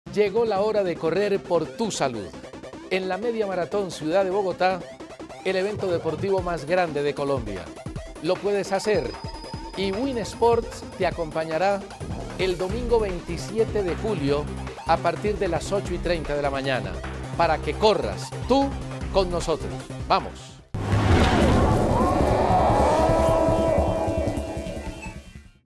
¡Es la hora de correr!- Win Sports
Win Sports te trae la carrera más importante de Colombia, este domingo 27 de julio no te pierdas la Media Maratón de Bogotá.